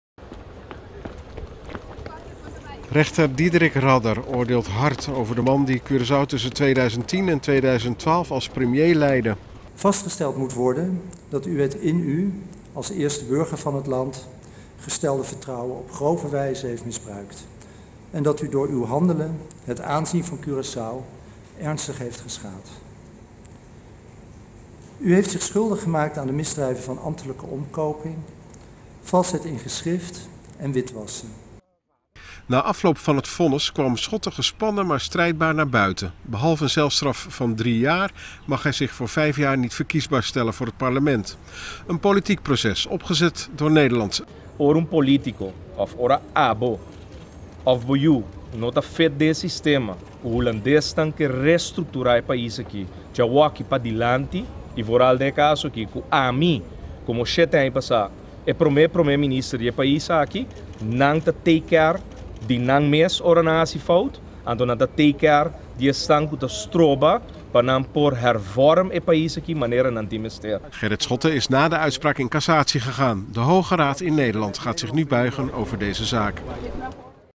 NOS: Uitspraak in hoger beroep in strafzaak Gerrit Schotte (Babel)
WILLEMSTAD (PBC) - Op vrijdag 21 juli 2017 heeft het Hof in hoger beroep uitspraak gedaan in de zaak van de verdachten Gerrit Schotte en Cicely van der Dijs. Gerrit Schotte is veroordeeld tot gevangenisstraf voor de duur van drie jaar met aftrek van voorarrest. Ook is hij ontzet uit het recht in de Staten gekozen te worden voor een periode van vijf jaar. Cicely van der Dijs, zijn levenspartner is veroordeeld tot een gevangenisstraf van vijftien maanden, met aftrek, waarvan zes maanden voorwaardelijk.